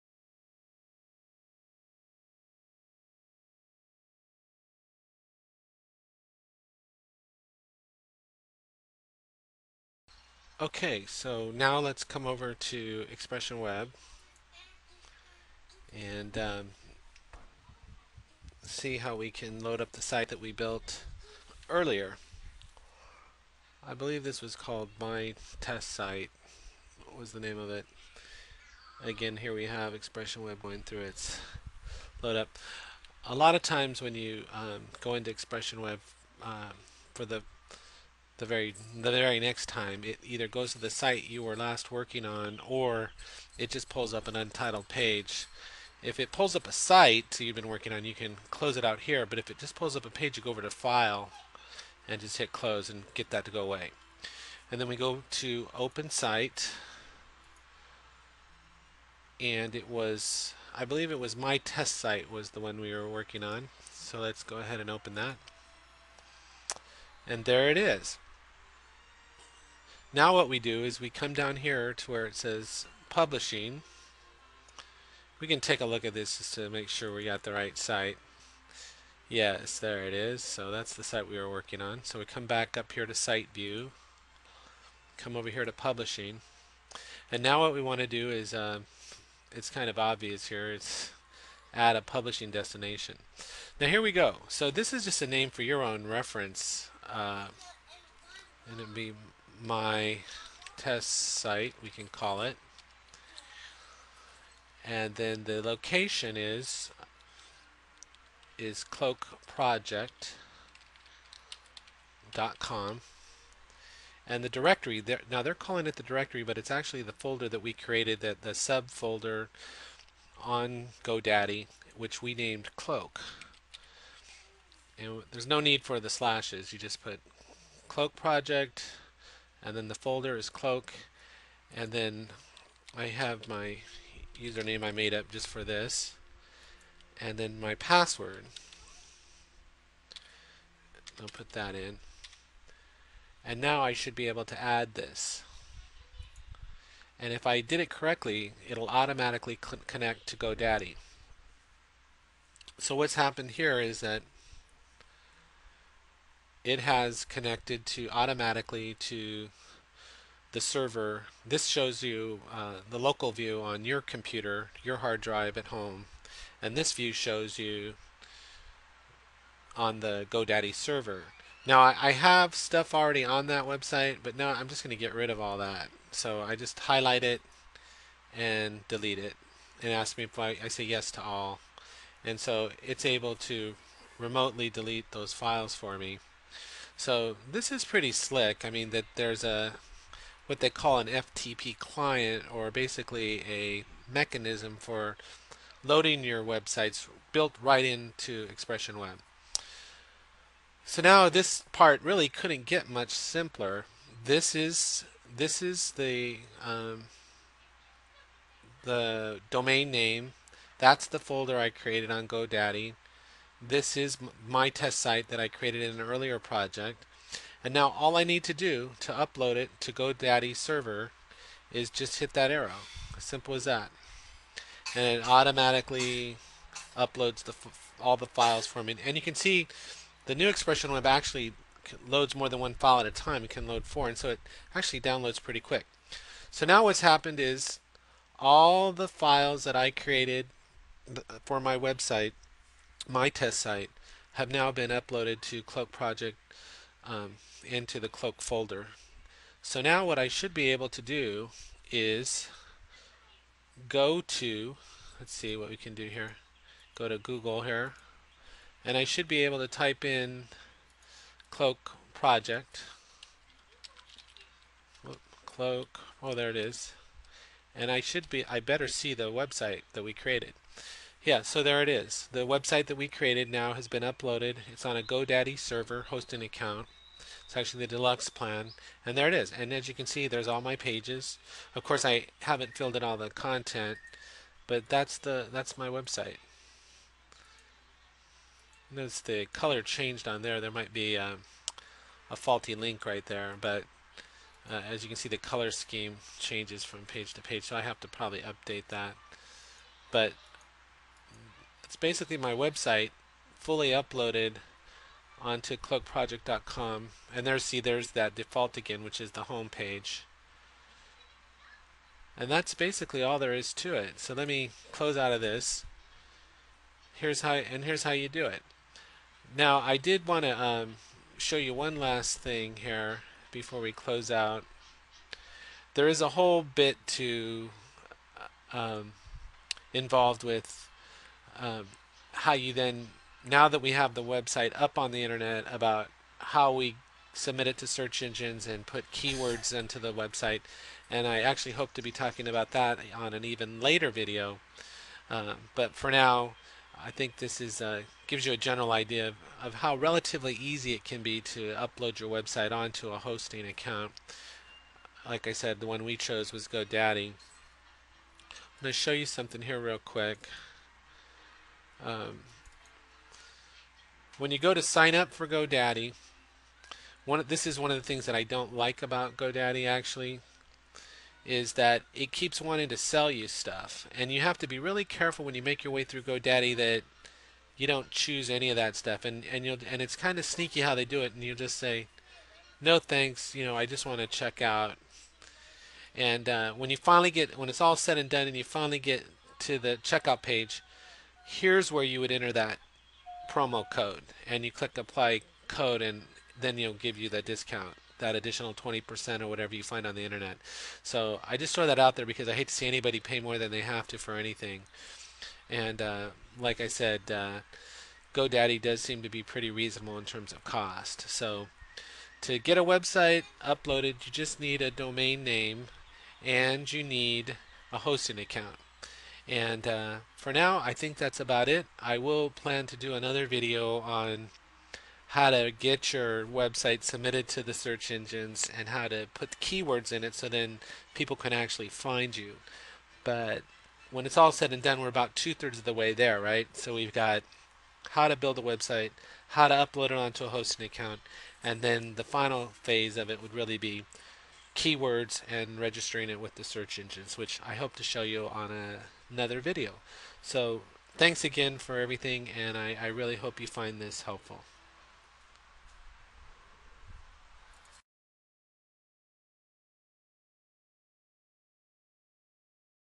Part 3 : Uploading Multiple Websites To A Hosting Account Using Go Daddy and Expression Web 3.0
A tutorial on how to upload multiple websites to a single hosting account using GoDaddy.com and Expression Web 3.0. This is part 3.